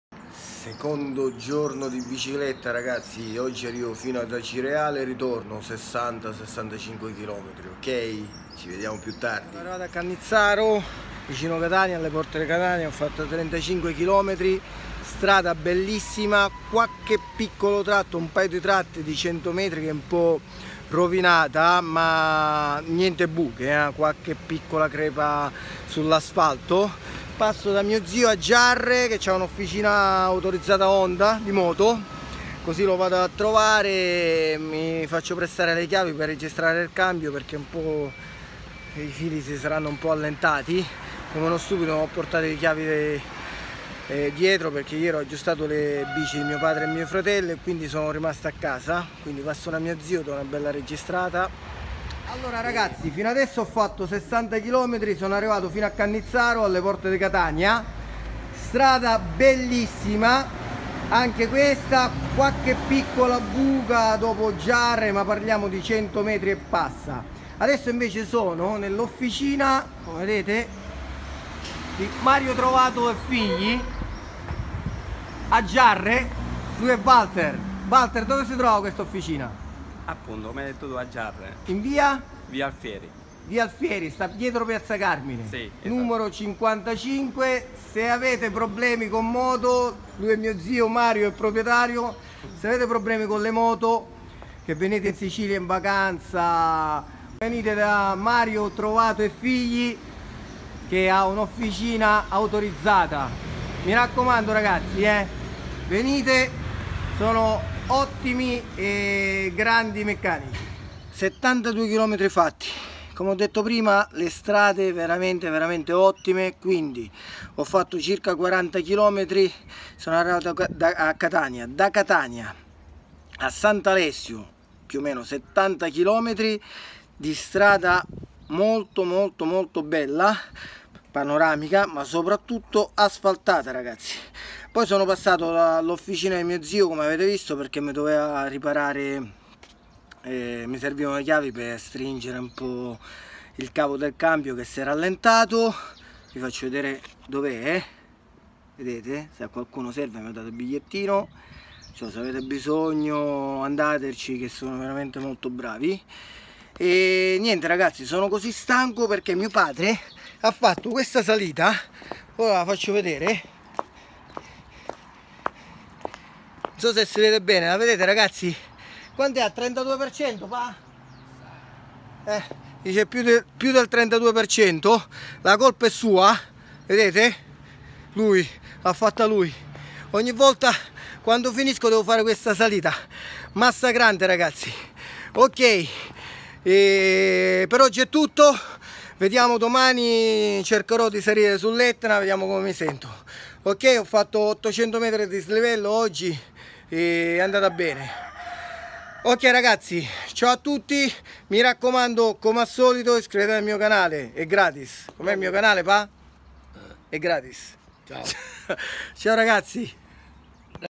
5^ giorno in Sicilia 2 ^ in bici
Ragazzi in Sicilia le strade sono asfaltate 72 km senza buche